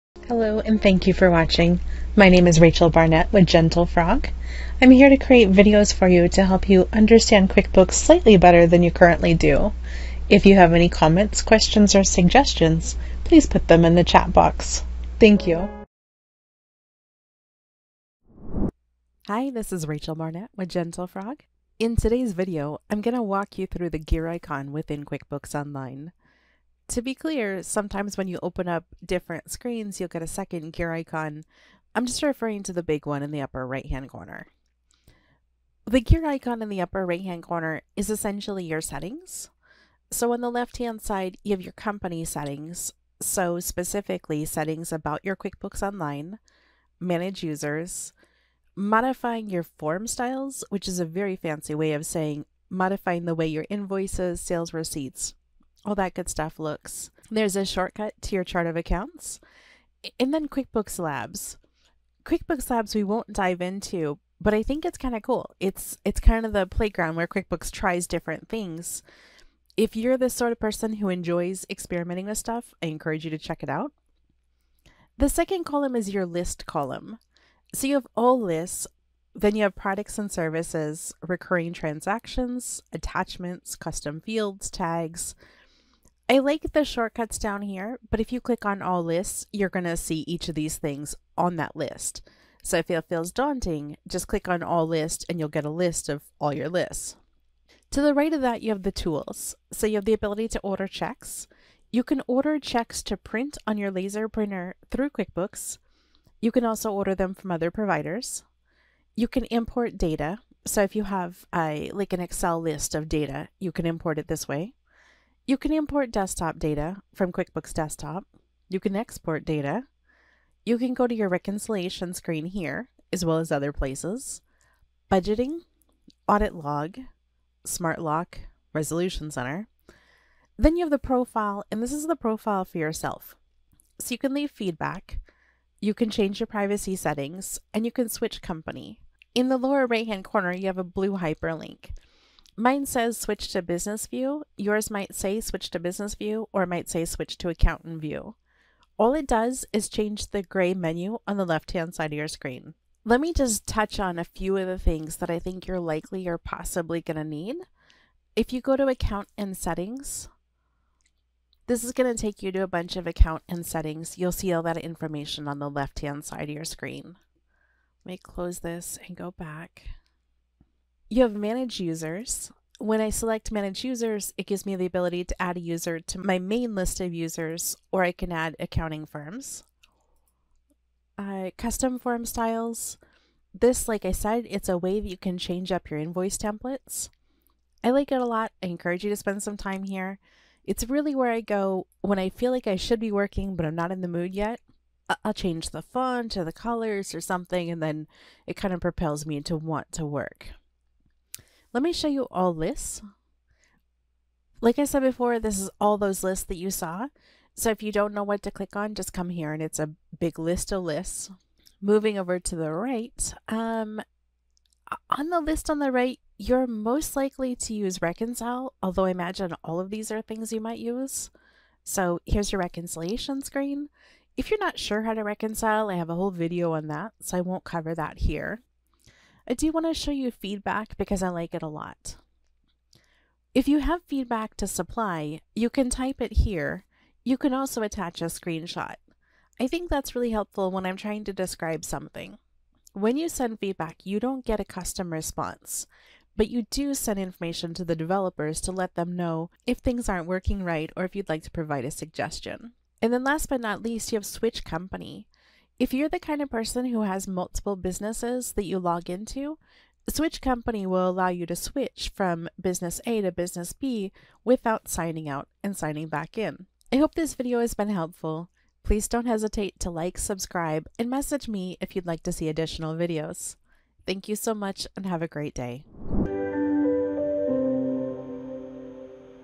QuickBooks Online Gear Icon Settings Menu Explained
In this video, I explain what you'll find when you click the gear icon in the upper right corner of QuickBooks Online.  This opens up the settings menu.

We'll go over what you'll find in the settings menu and some of the most common places small businesses might go within the settings. This includes Account and settings, Manage users, Custom from styles, All lists, Reconcile, Feedback, and Switch company.

Timestamps
00:00 - Intro
00:22 - Gear icon/settings menu
00:40 - Your company
01:14 - Lists
01:37 - Tools
02:10 - Profile
02:18 - Switch view
02:32 - Account and settings
02:50 - Manage users
03:00 - Custom form styles
03:20 - All lists
03:33 - Reconcile
03:48 - Feedback
04:11 - Switch company
04:26 - Outro